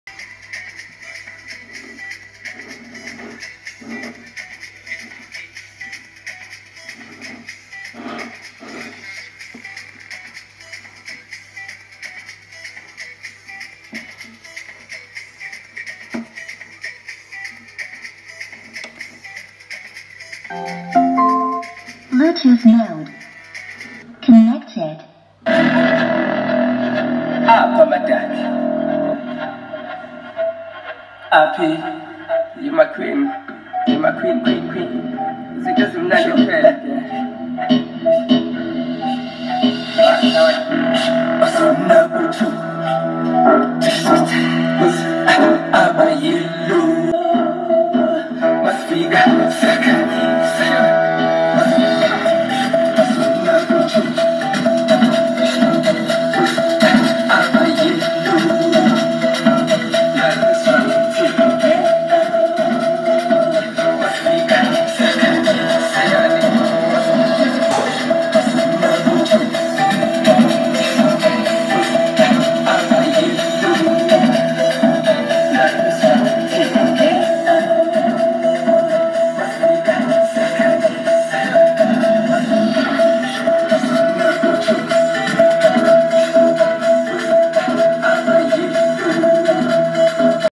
2022 Picglow sound speaker |bluetooth speaker|lithophane lamp
022 Picglow sound speaker |bluetooth speaker|lithophane lamp

Get a picglow sound speaker for your partner as a present. Futures include Bluetooth connection, aux connection, disco lights for partying, usb connection,rechargeable-lamp

Order takes 3-7days anywhere around south africa